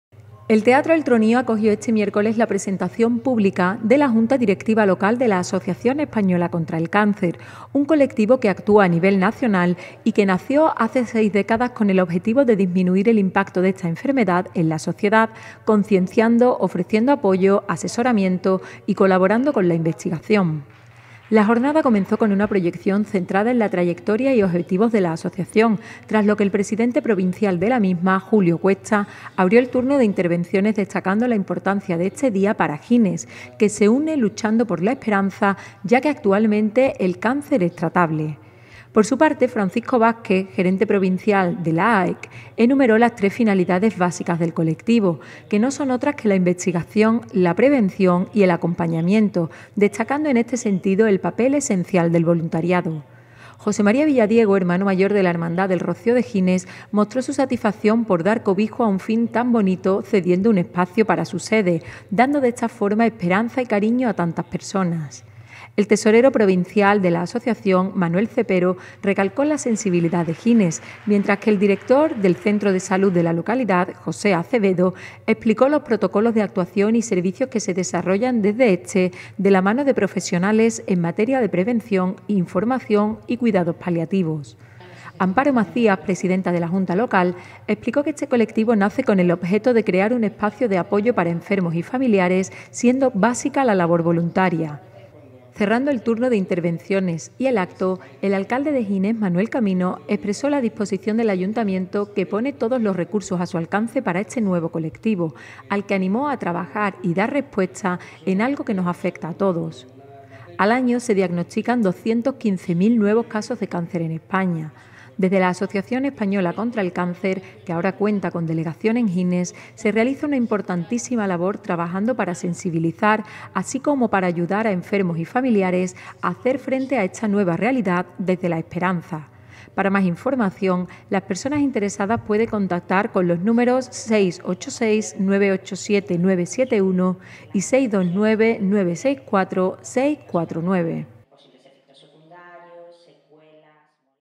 Presentada la Junta directiva de Gines de la Asociación Española Contra el Cáncer (AECC)
El teatro “El Tronío” acogió este miércoles la presentación pública de la junta directiva local de la Asociación Española Contra el Cáncer (AECC), un colectivo que actúa a nivel nacional y que nació hace 6 décadas con el objetivo de disminuir el impacto de esta enfermedad en la sociedad concienciando, ofreciendo apoyo, asesoramiento y colaborando con la investigación.